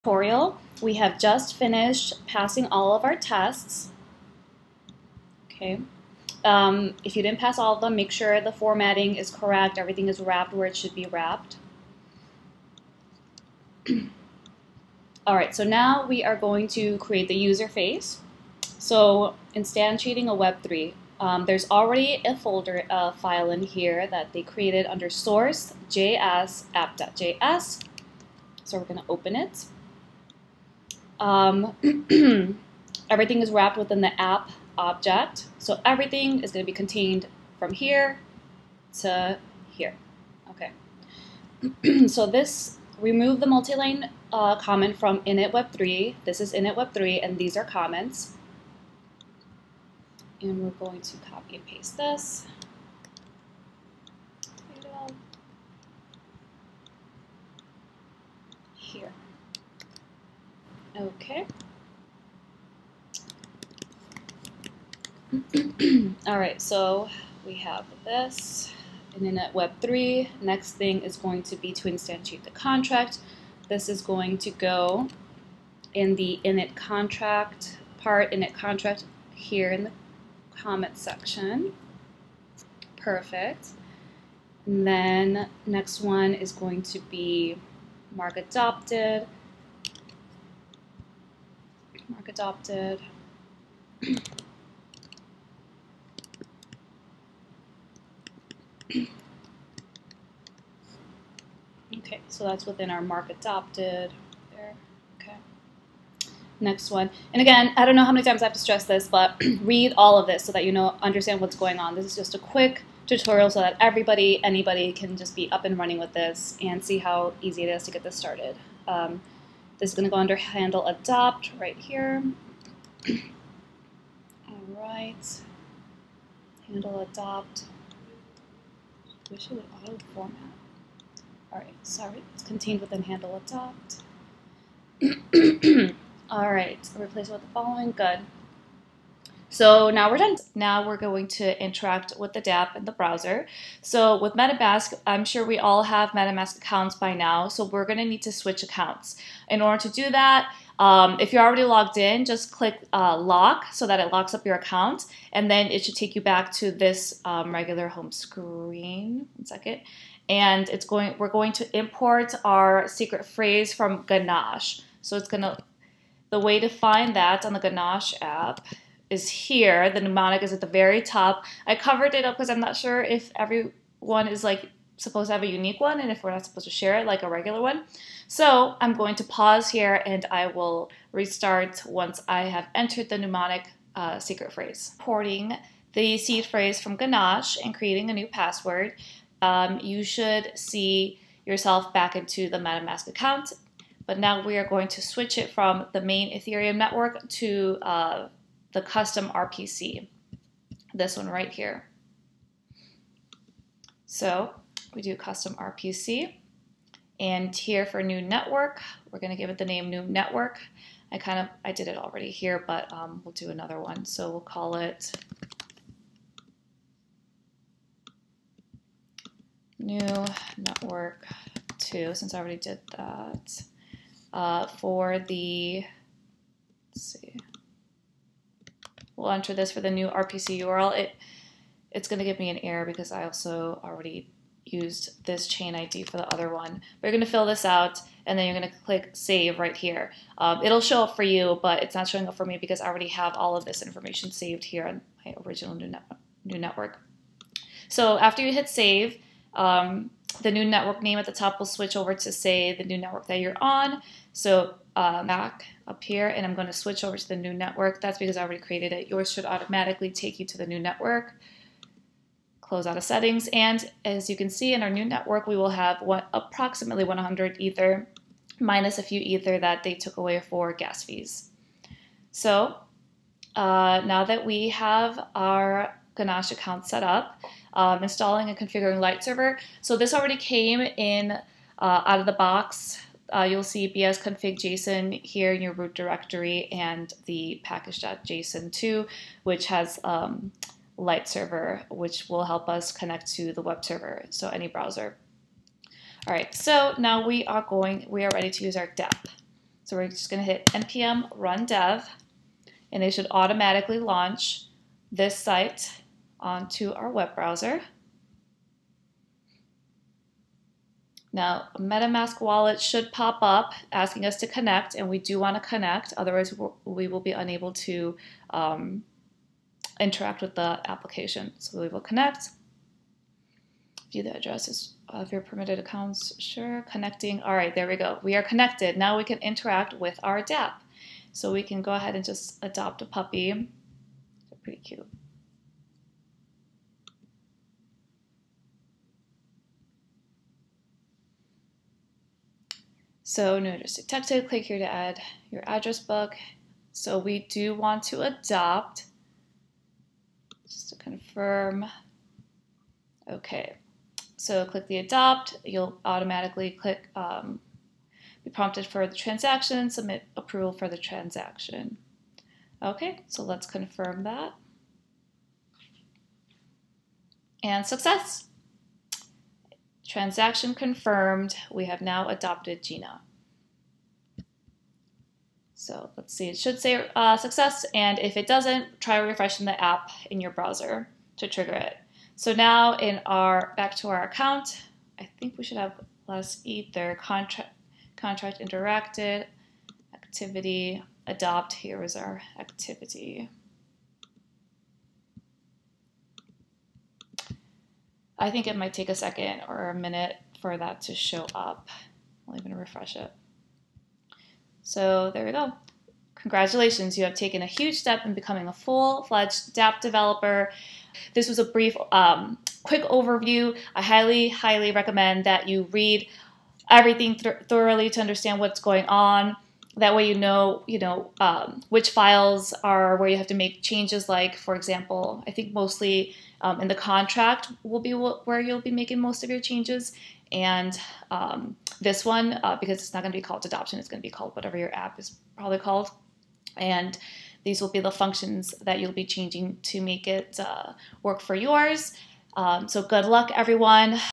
Truffle Pet Shop Tutorial - Part 2
Quick guided tutorial setting up the Truffle Pet Shop Tutorial, covering sections 6 - 7:
6. Creating a user interface to interact with the smart contract
7. Interacting with the dapp in a browser

Meant to help users with little to no technical background interact with their dApp in the browser quickly.  Assumes you have already watched Part 1